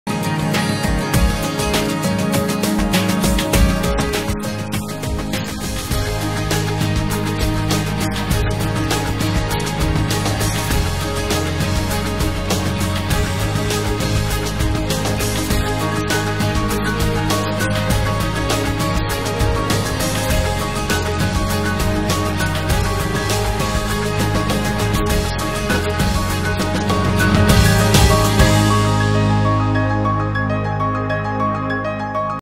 Международный турнир по бадминтону "Babolat Kharkov International - 2014"
5-7 сентября 2014 г. во Дворце спорта "Локомотив" пройдет международный турнир по бадминтону "Babolat Kharkov International - 2014"